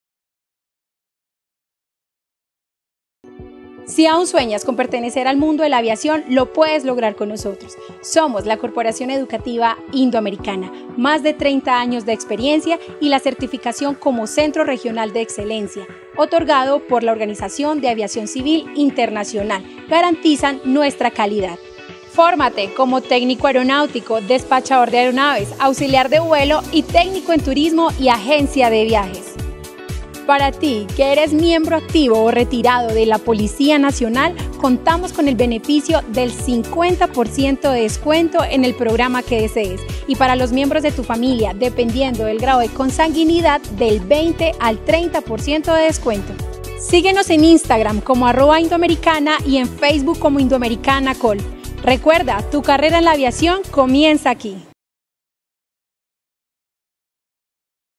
Convenio Educativo Policía Nacional
Conoce la alianza que tenemos con la Policía Nacional, para la formación de técnicos laborales enfocados en desempeñarse en el sector aeronáutico.